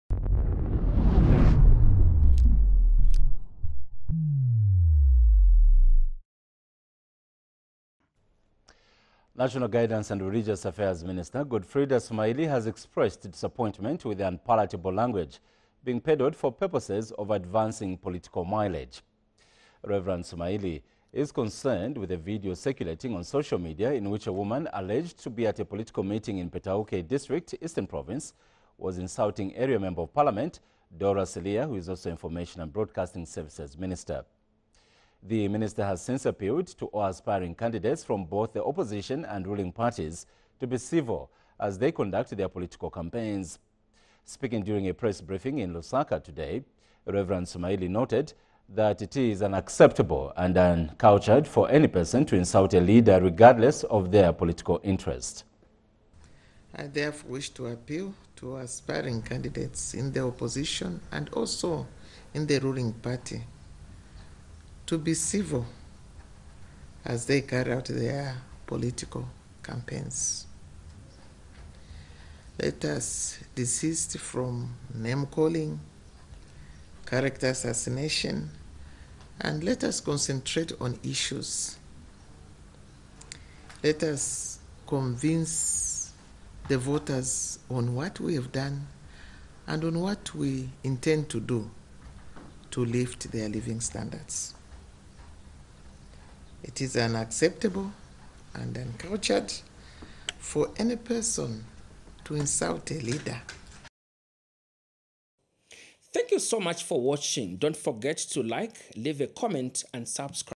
SUMAILI CONDEMNS UNPALATABLE LANGUAGE
National Guidance and Religious Affairs Minister Godfridah Sumaili has expressed disappointment with the unpalatable language being peddled for purposes of advancing political mileage.

Reverend Sumaili is concerned with a video circulating on social media in which a woman alleged to be at a political meeting in Petauke District - Eastern Province was insulting area Member of Parliament Dora Siliya  who is also Information and Broadcasting Services Minister.